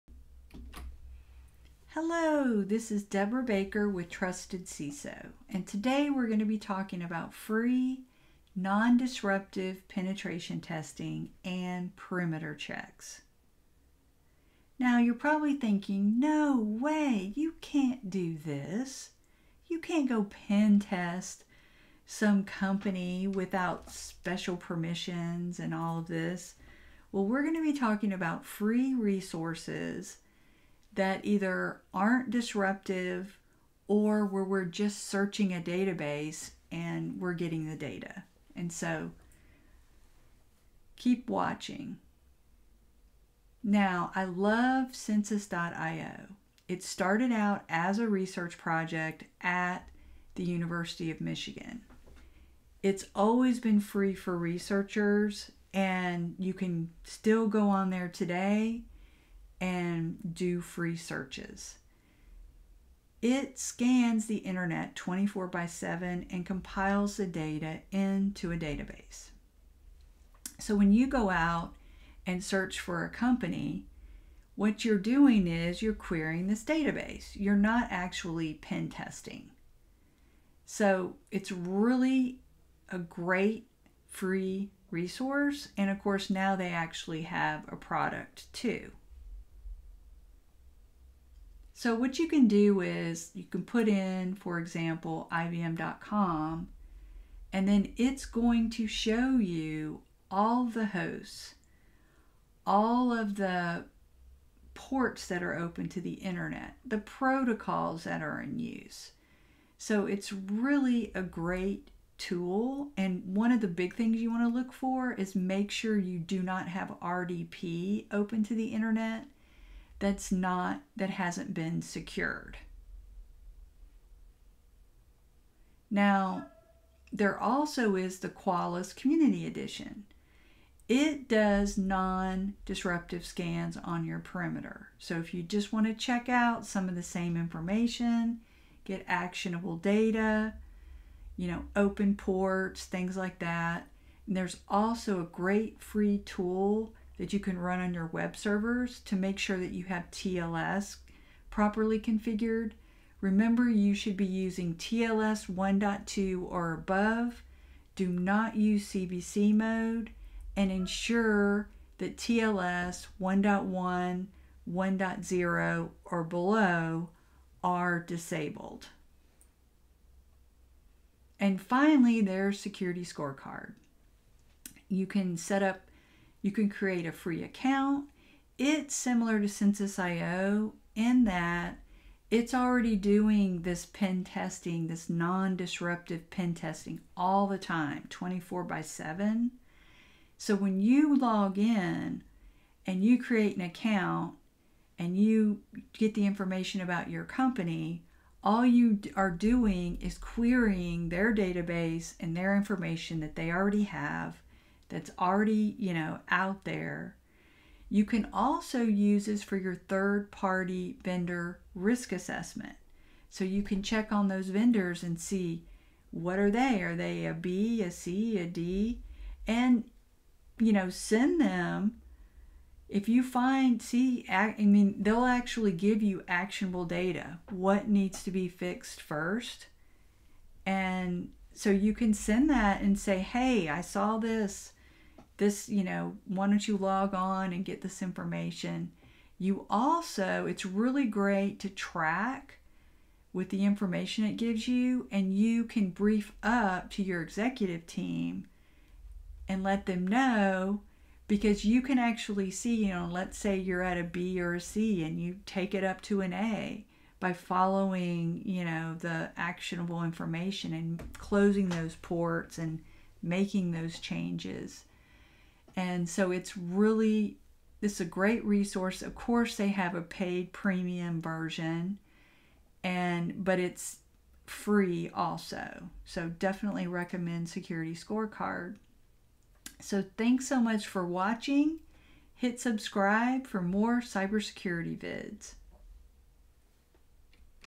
Non disruptive Pen tests & Free tools
✅ CISO Guide to Cyber Resilience Book

In this video I describe non-disruptive penetration testing and perimeter checks that you can do with Free tools.